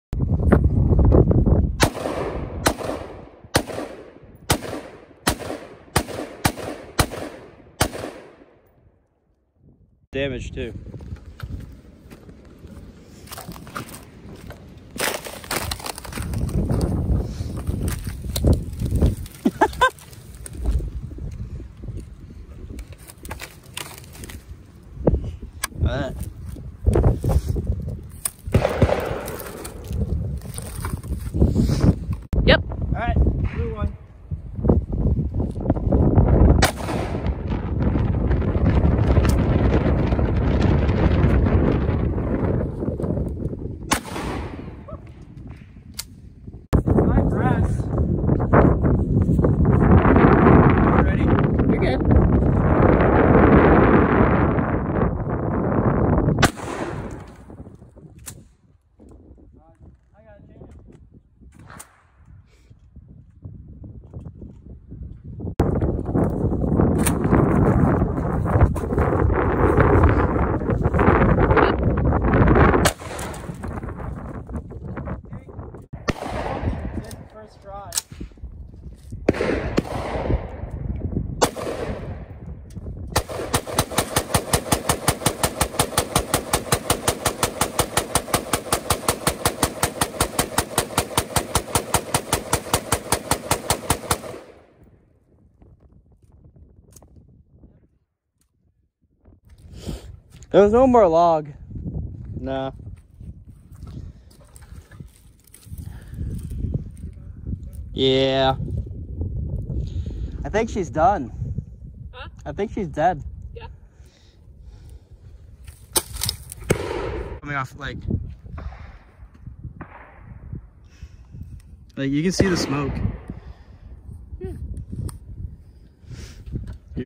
Range Day Alaska
This is a compilation of the last time I went to the range while I lived in Alaska, Enjoy!